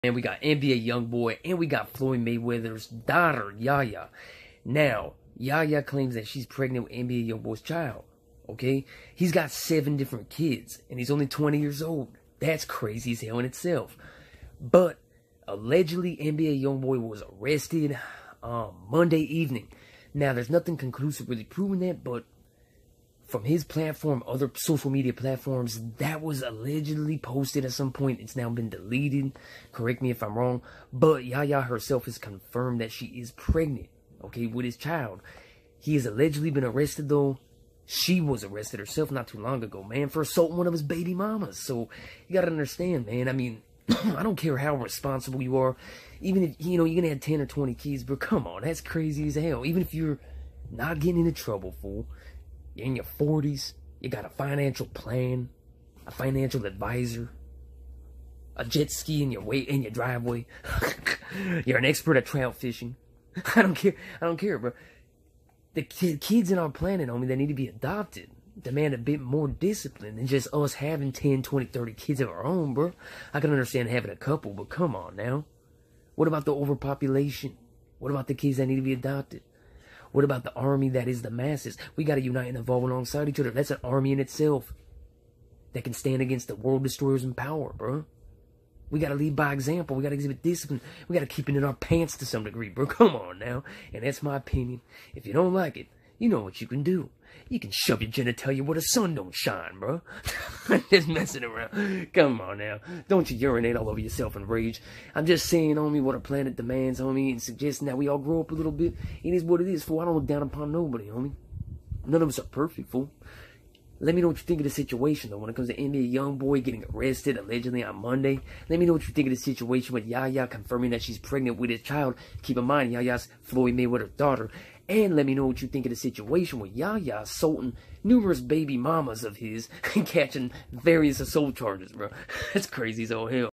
NBA YoungBoy Allegedly Arrested & Floyd Mayweather's Daughter Yaya PREGNANT With HIS Child!?
NBA YoungBoy was Allegedly arrested Monday evening & Floyd Mayweather's Daughter Yaya is Allegedly pregnant with NBA YoungBoy's child! YoungBoy is now 20 years old & currently has 7 children if I'm correct..at least 7!